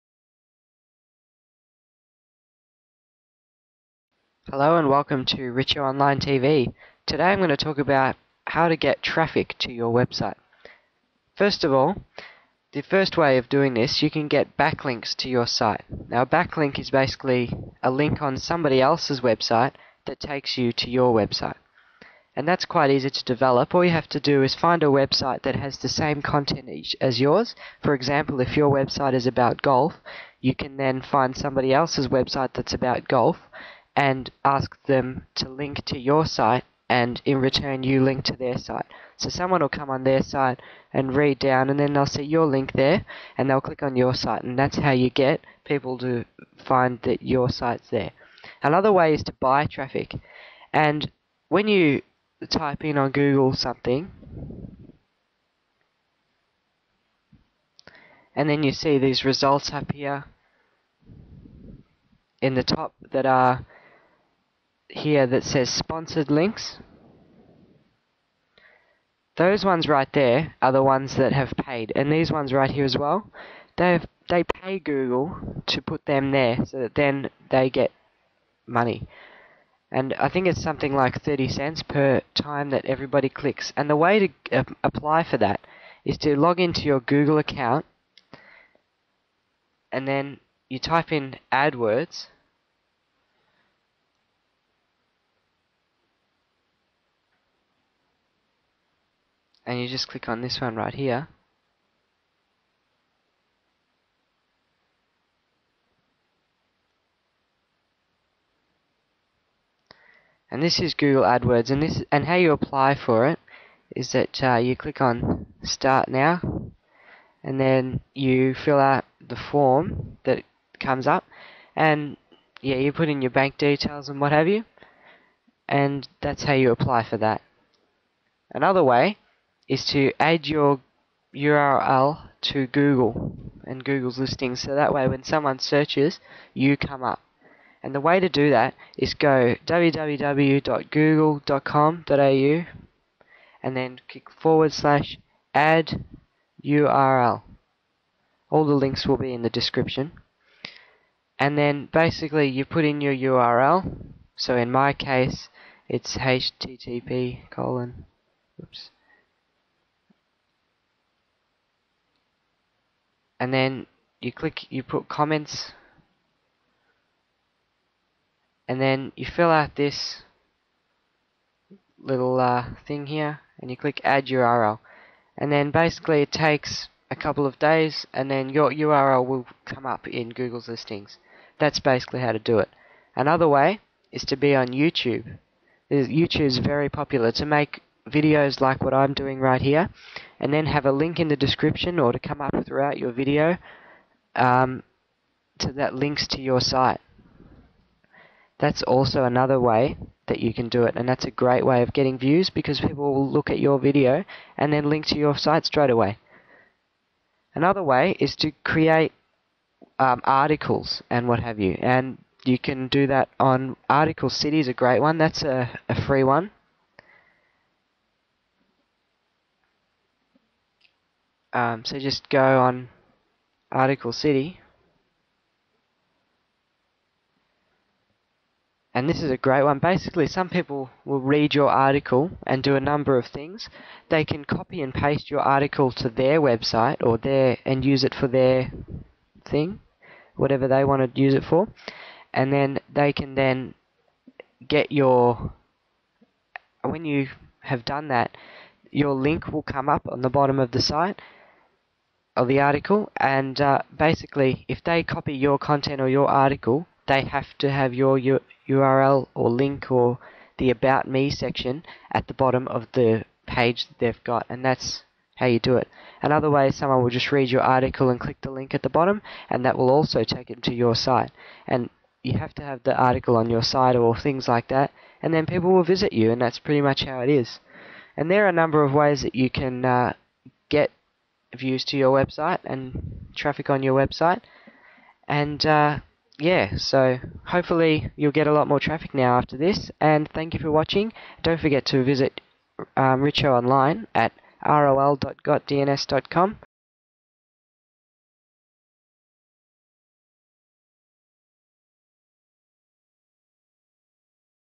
How to get Traffic on your Website
In this video Richo Online Tv shows you the best ways to get more website traffic. Follow these steps to make your website more popular.